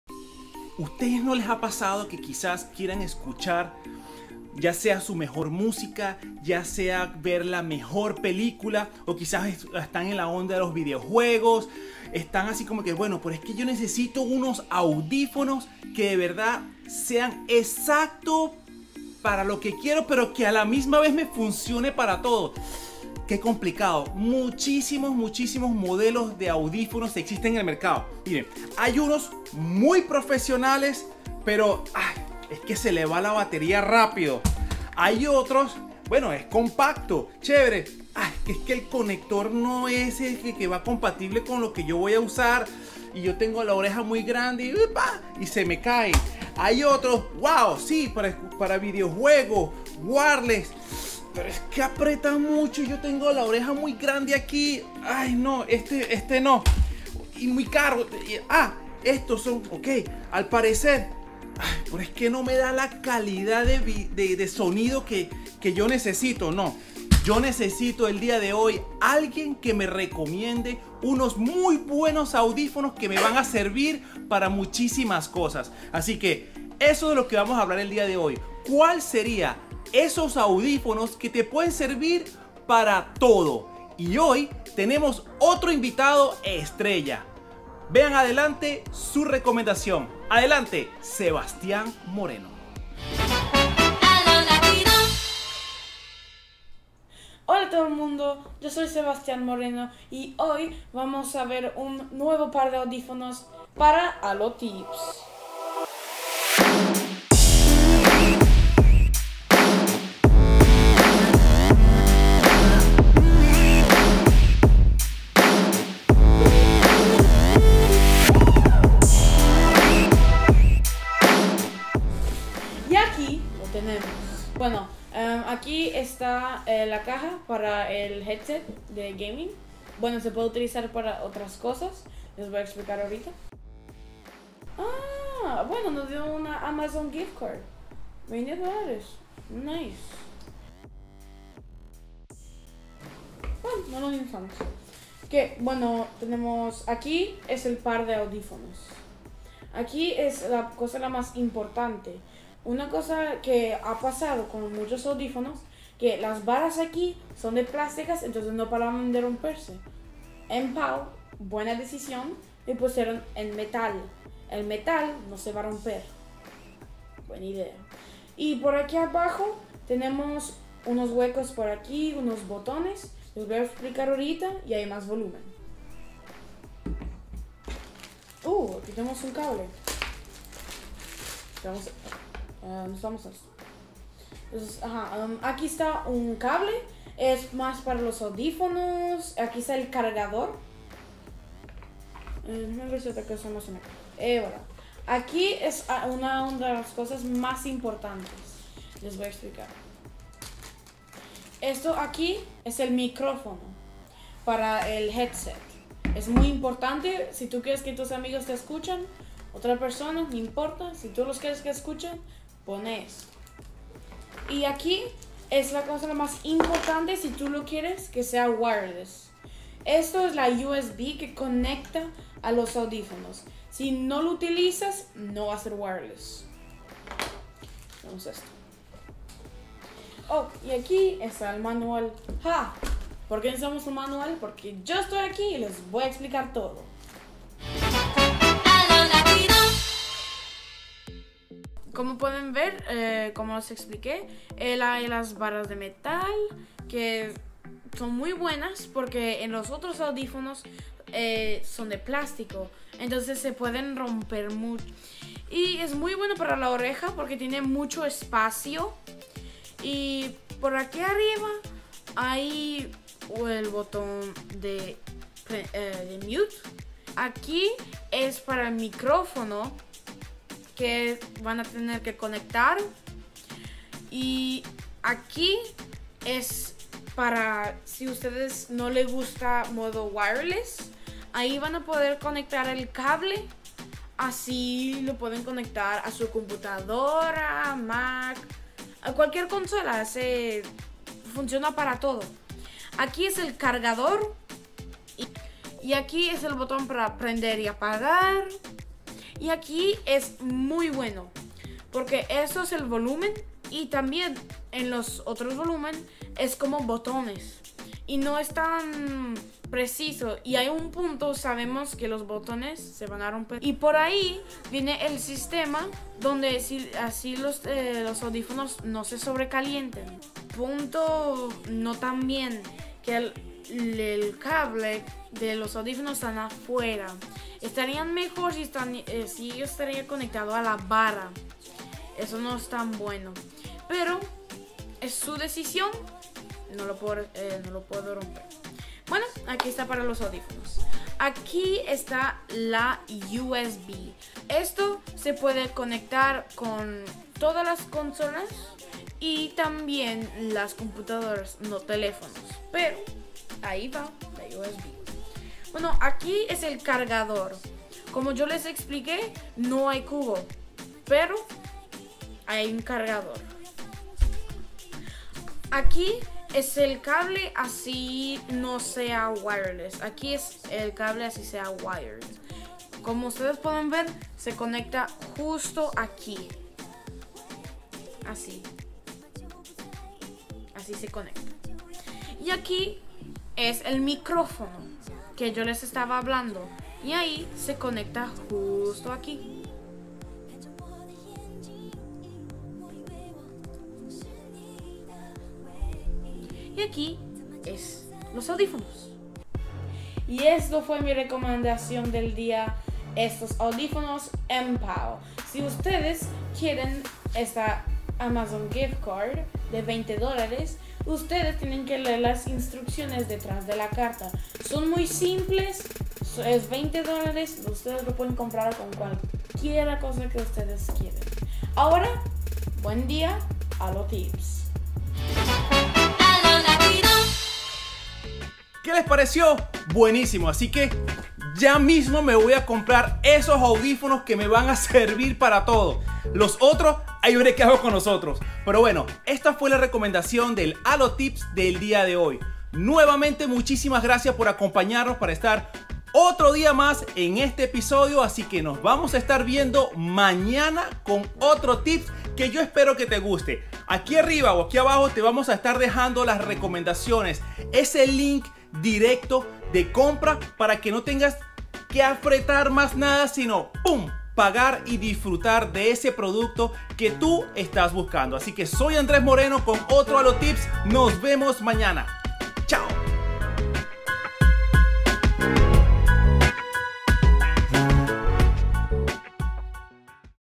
ALO TIPS - MPOW Headphone Wireless 2.4GHz
@alolatinotv - Tantos audifonos para cada cosa, pero aqui @soysebastianmoreno Nos trae uno de alta calidad y buen precio.
LINK DE COMPRA DIRECTA:
CONCURSO ACTIVO. Instrucciones en el link de la biografía.
ACTIVAR LA CAMPANITA para que te lleguen las notificaciones.